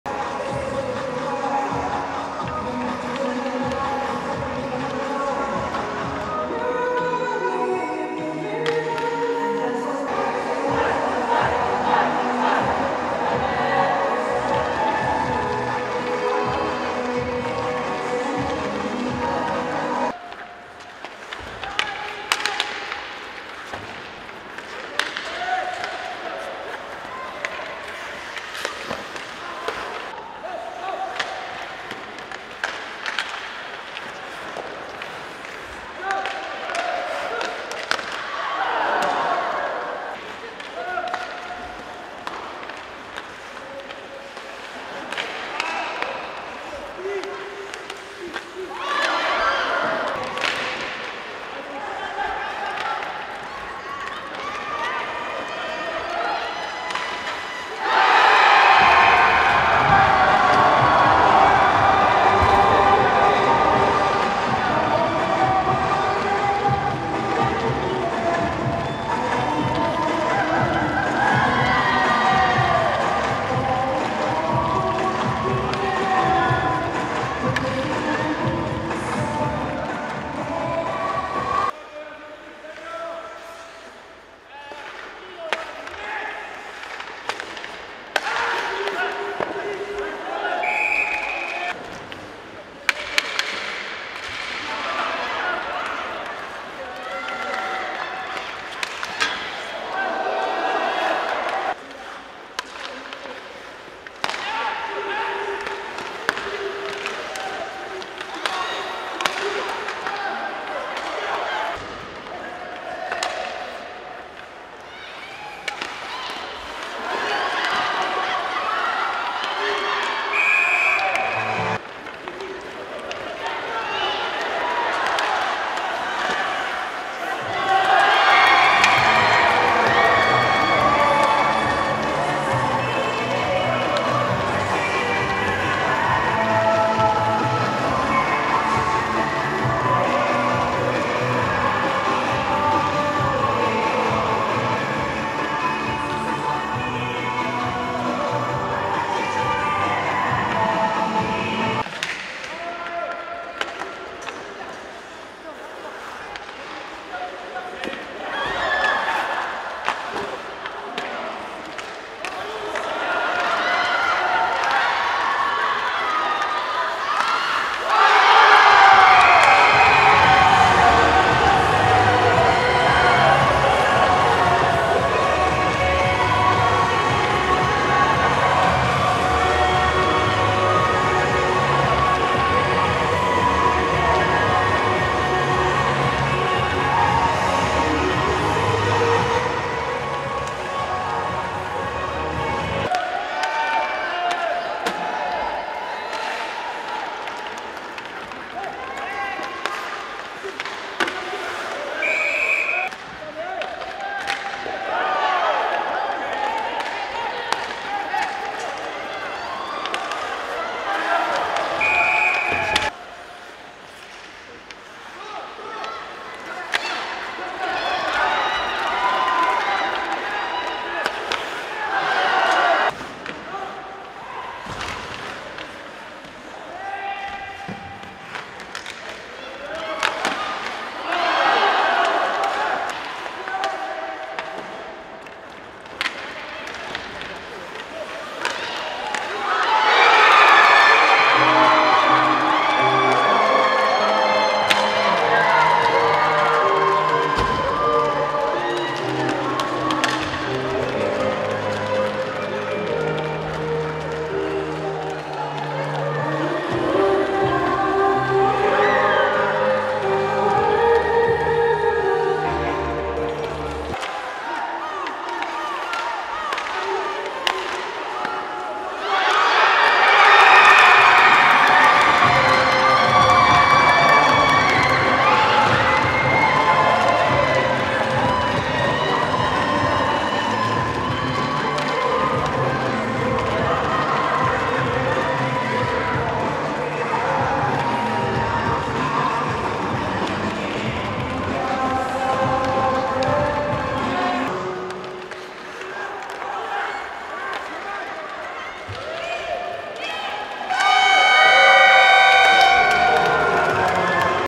하이라이트 | 광운중 vs 중동중 | 2024 유청소년 주말리그 중등부 A권역 | 2024. 7. 27
2024 유청소년 주말리그 중등부 A권역
광운중 vs 중동중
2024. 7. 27, 강릉하키센터

주최 : 대한체육회
주관 : 대한아이스하키협회
후원 : 문화체육관광부
재정지원 : 국민체육진흥공단